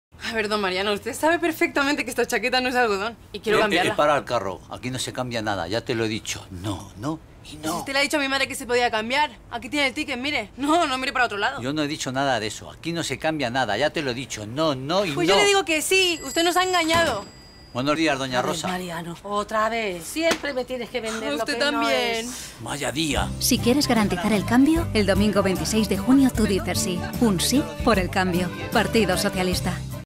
Polémica campaña del PSOE para el 26J
Comerciantes de toda España piden al partido que lo retire ya que considera que desprecia al sector.